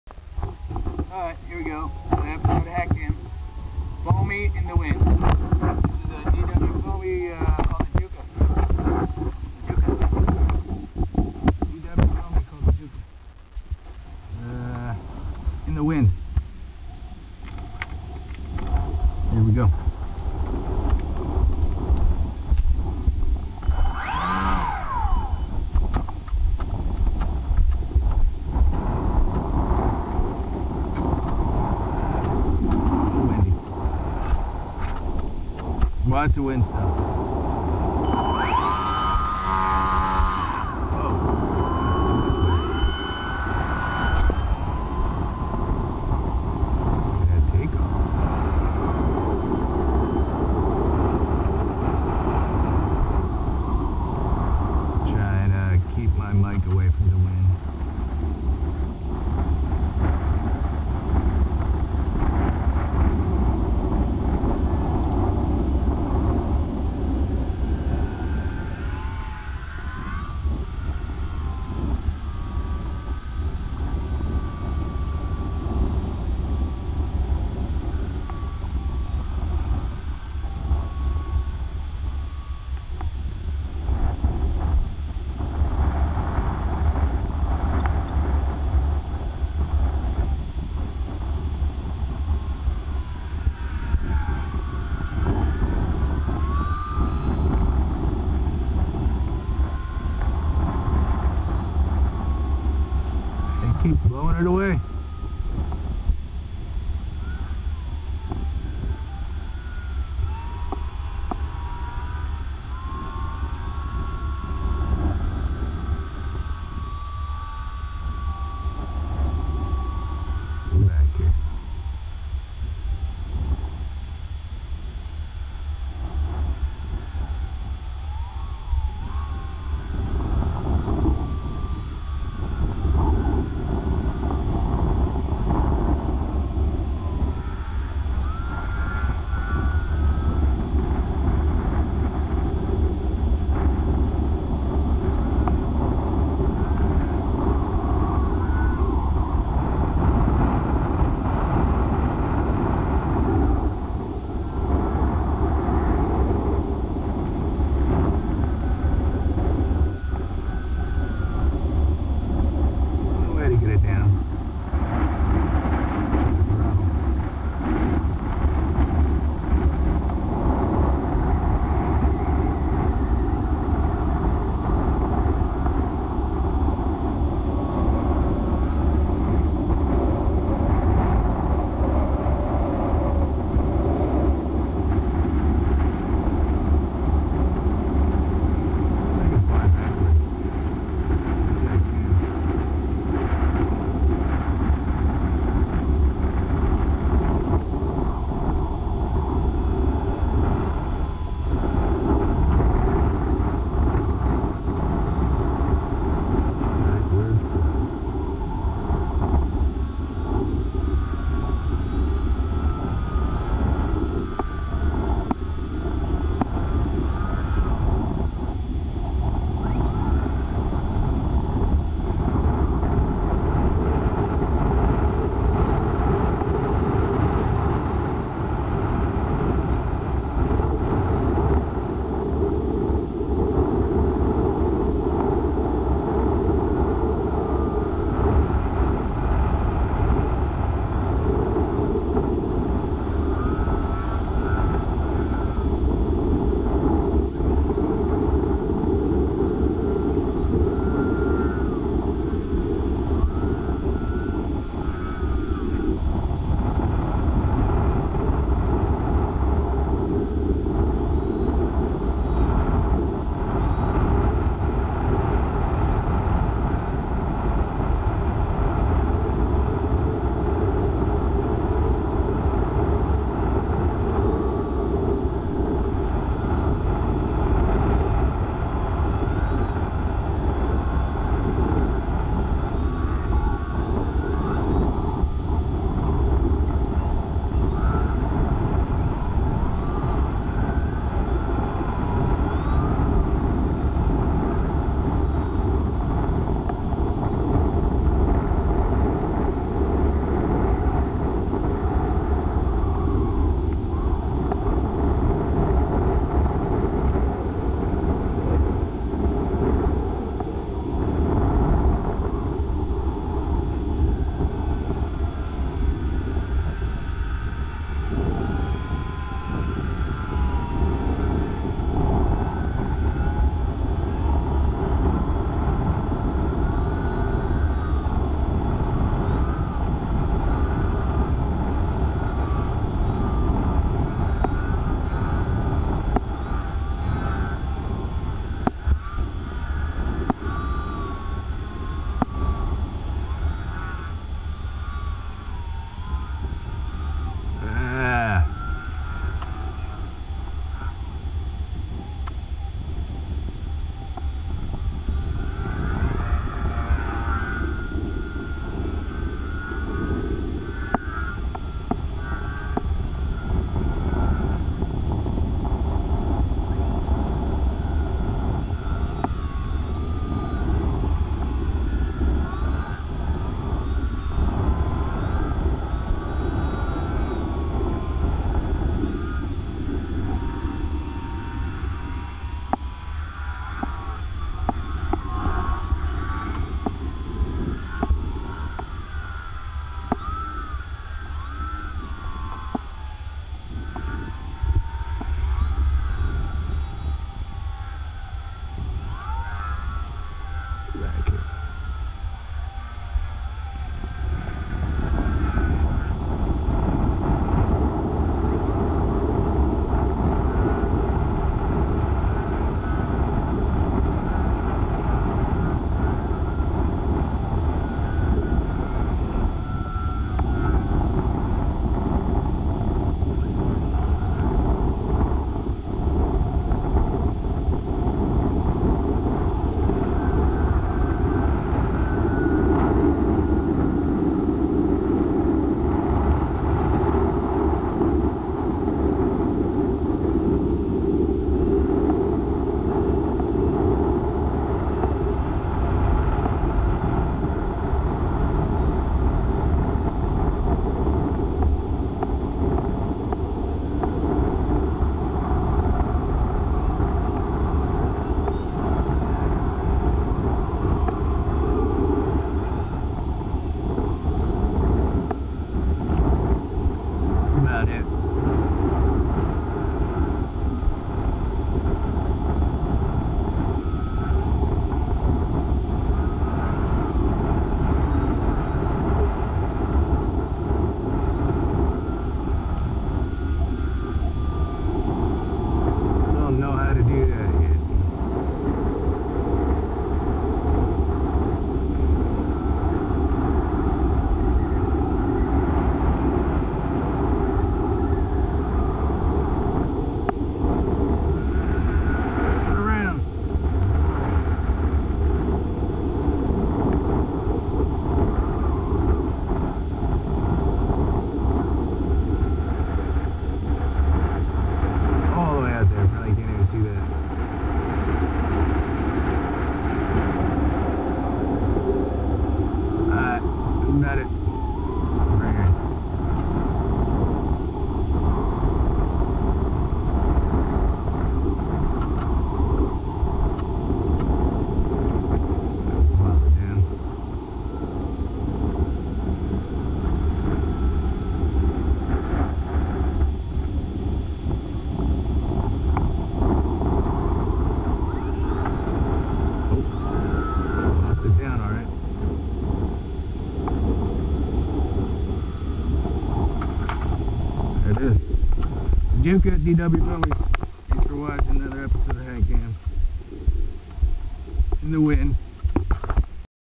learning to fly in the wind DW foamy Juka
Learning to fly in the wind, You tell me what the wind speed is? who knows